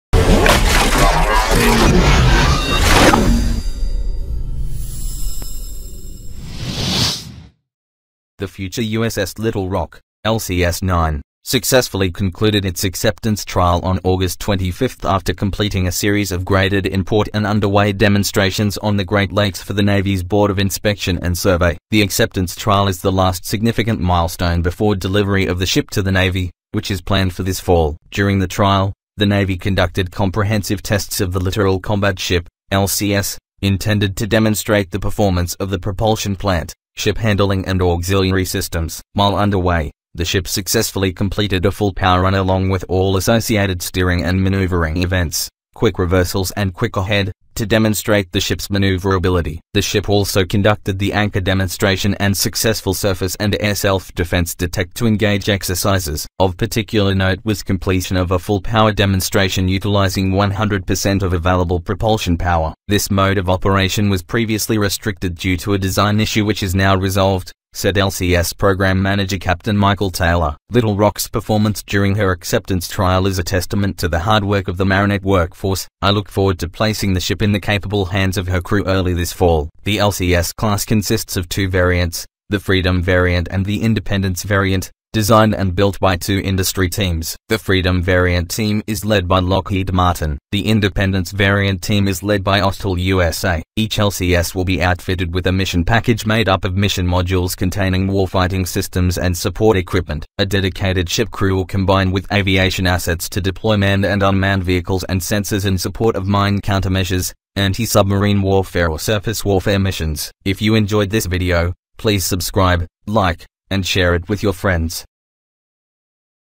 USS Little Rock (LCS9) Littoral Combat Ship Concludes Acceptance Trial, US Navy Will Accept Delivery
The future USS Little Rock (LCS 9) successfully concluded its acceptance trial on August 25 after completing a series of graded in-port and underway demonstrations on the Great Lakes for the Navy's Board of Inspection and Survey.

The acceptance trial is the last significant milestone before delivery of the ship to the Navy, which is planned for this fall. During the trial, the Navy conducted comprehensive tests of the Littoral Combat Ship (LCS) intended to demonstrate the performance of the propulsion plant, ship handling and auxiliary systems. While underway, the ship successfully completed a full-power run along with all associated steering and maneuvering events (quick reversals and quick ahead) to demonstrate the ship's maneuverability. The ship also conducted the anchor demonstration and successful surface and air self-defense detect-to-engage exercises.

"Of particular note was completion of a full-power demonstration utilizing 100 percent of available propulsion power. This mode of operation was previously restricted due to a design issue which is now resolved," said LCS Program Manager Captain Michael Taylor. "Little Rock's performance during her acceptance trial is a testament to the hard work of the Marinette workforce. I look forward to placing the ship in the capable hands of her crew early this fall."

The LCS class consists of two variants, the Freedom variant and the Independence variant, designed and built by two industry teams. The Freedom variant team is led by Lockheed Martin. The Independence variant team is led by Austal USA.

Each LCS will be outfitted with a mission package made up of mission modules containing warfighting systems and support equipment. A dedicated ship crew will combine with aviation assets to deploy manned and unmanned vehicles and sensors in support of mine countermeasures, anti-submarine warfare or surface warfare missions.